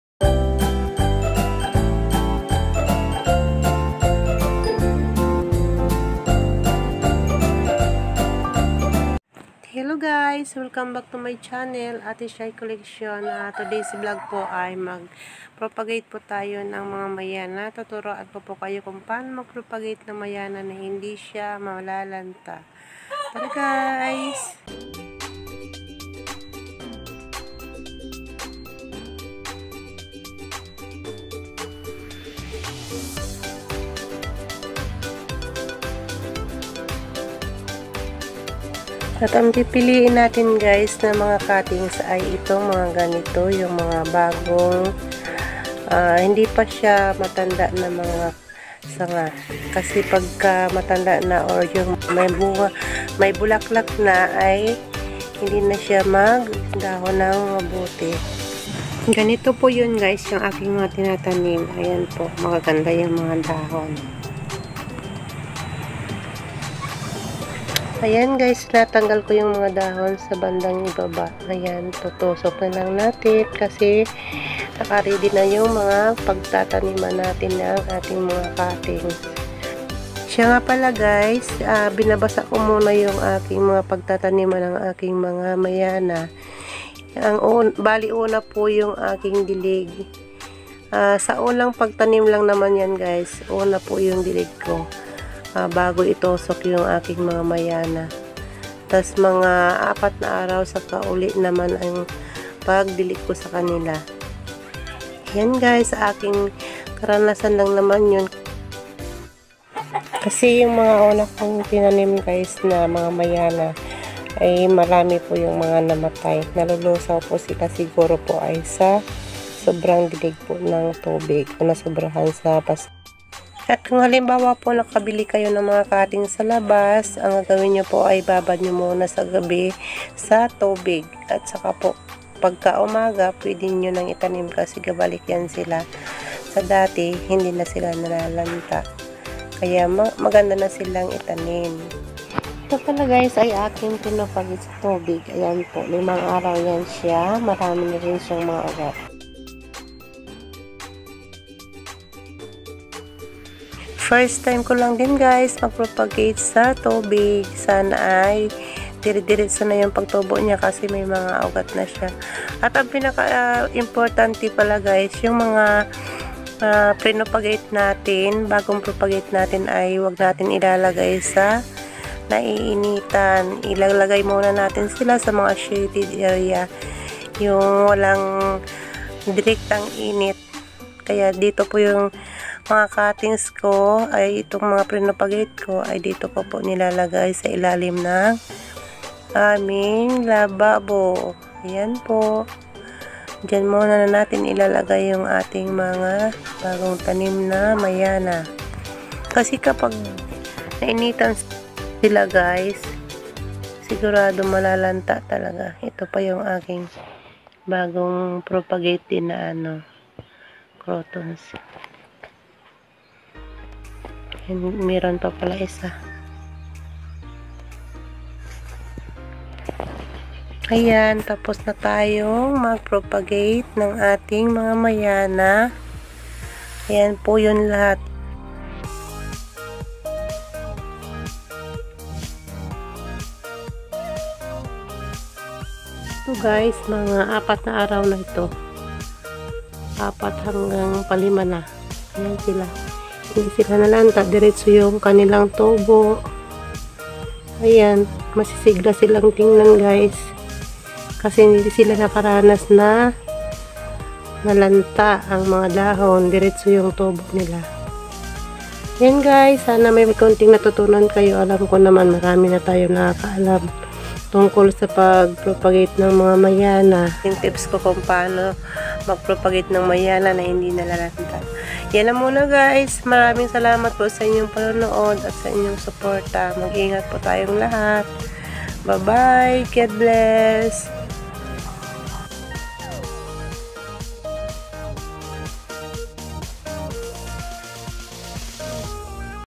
Pano magtanim ng Mayana na Hindi nalalanta panomagpropagate ng Mayana.
Gusto ko lang po ibahagi Kong pano magtanim ng Mayana na Hindi nalalanta..